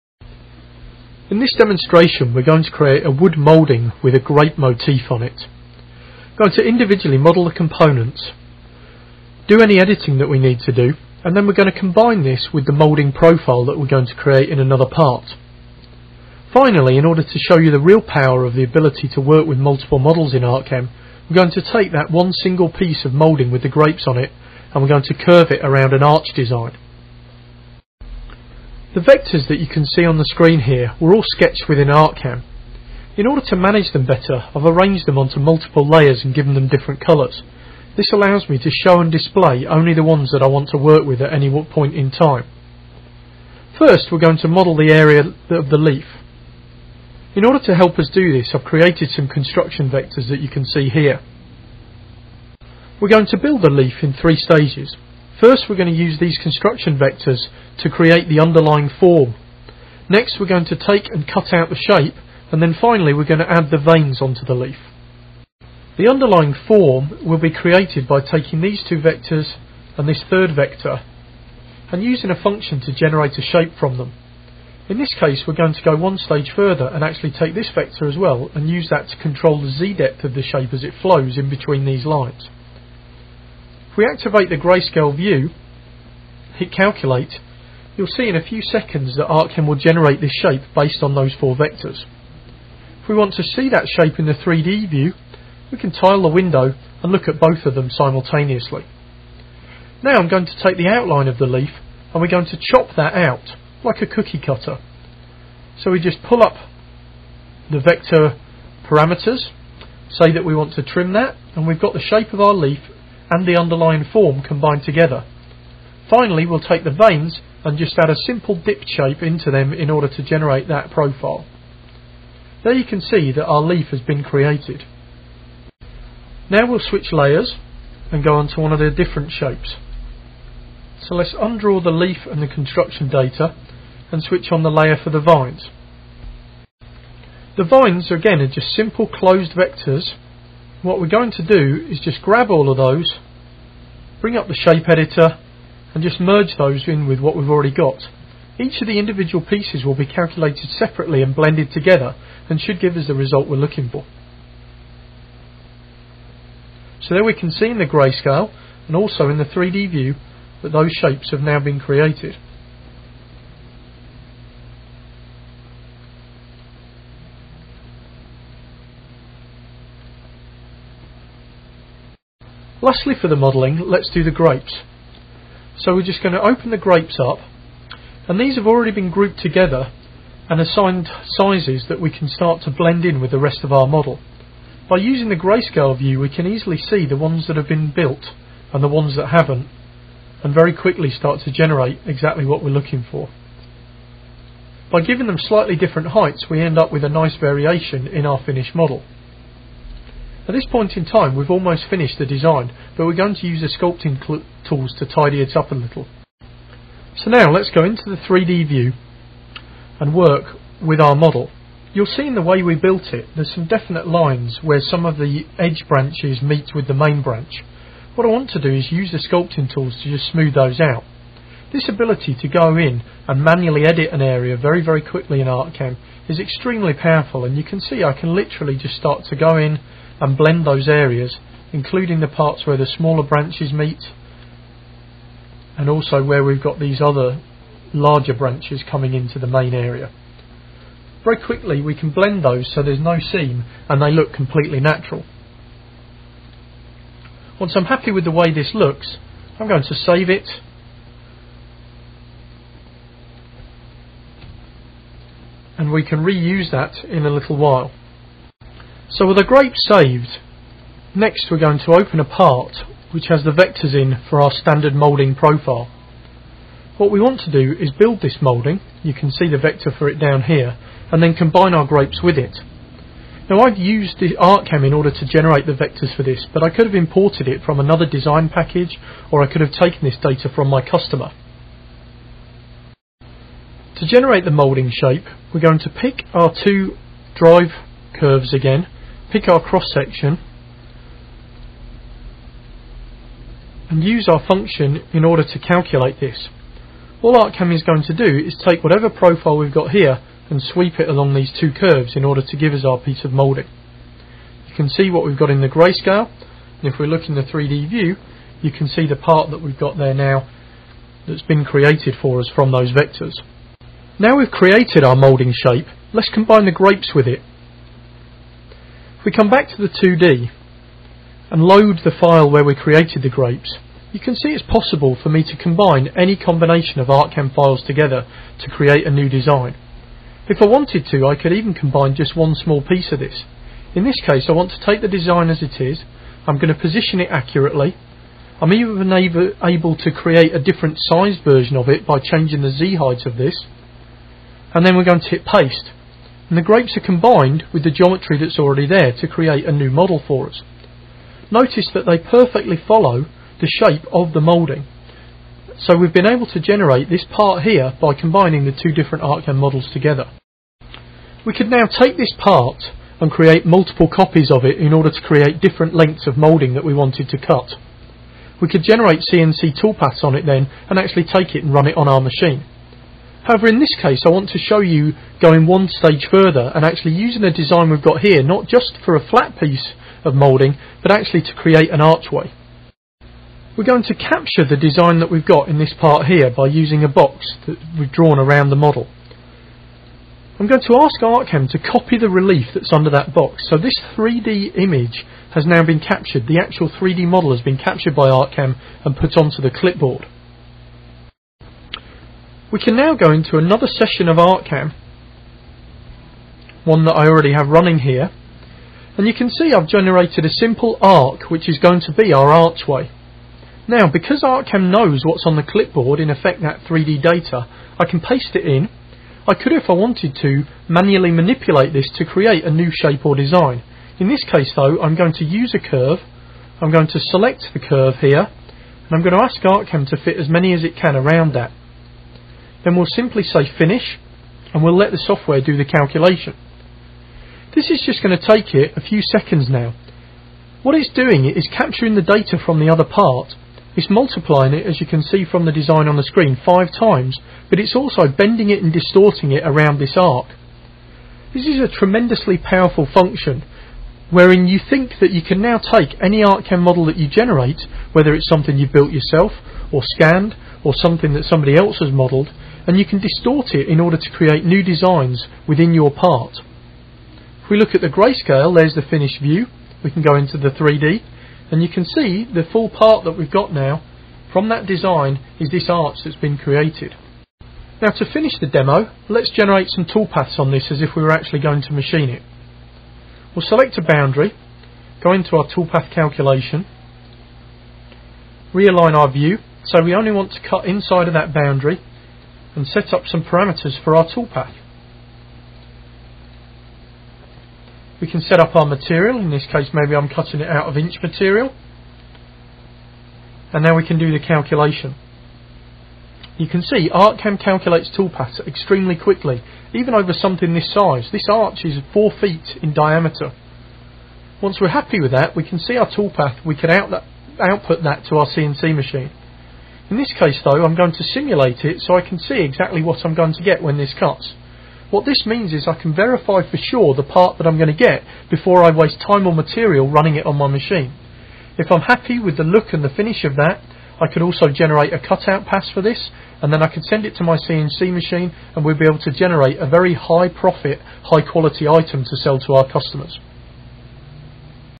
ArtCAM - Wood Moulding w/ Grape Motif - Curved Moulding
This video shows how to turn a 2D design into a 3D part using the ArtCAM Pro software. It also takes this 3D part and places it onto a fancy moulding background and then it gets bent into the final curved moulding.